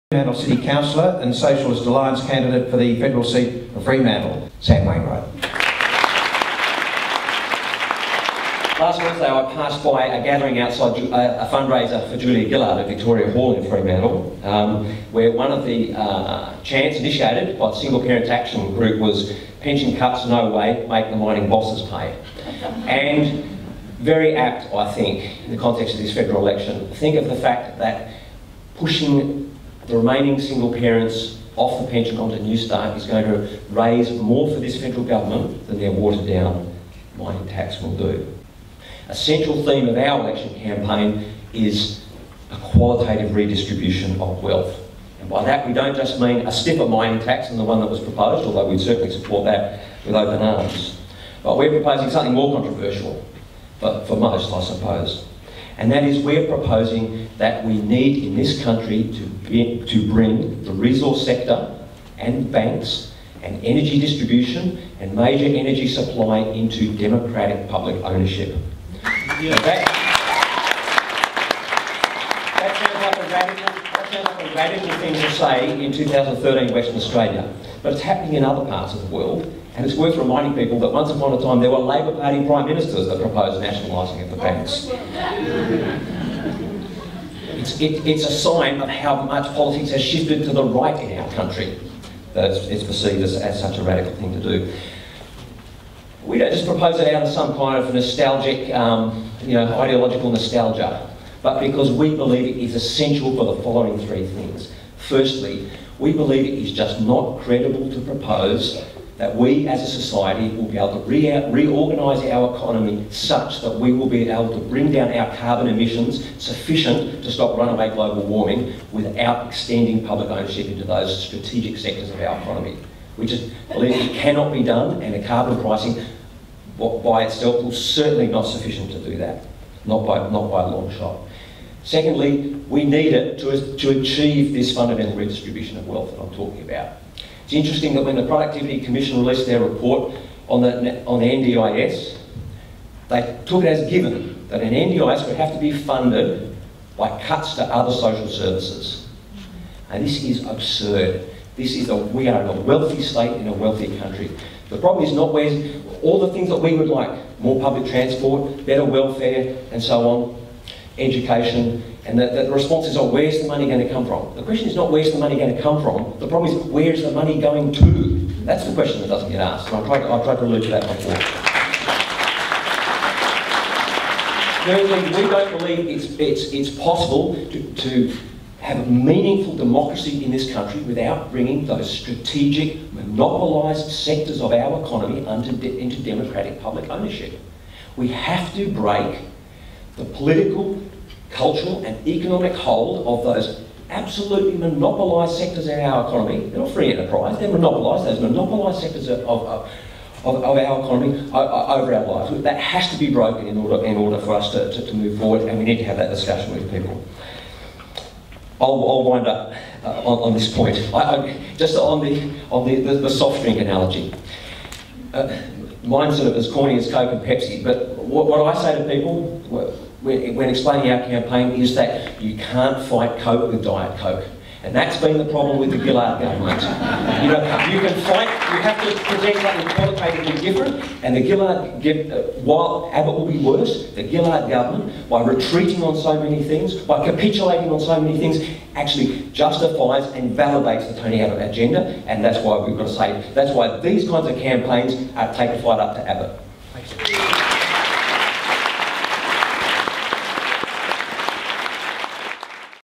Sam Wainwright: You can't fight coke with diet coke!
Socialist Alliance candidate for Fremantle, Sam Wainwright introduces the Socialist Alliance campaign to a packed out candidates forum organised by GetUp.

The forum was held in the state library theatre on 14 June 2013.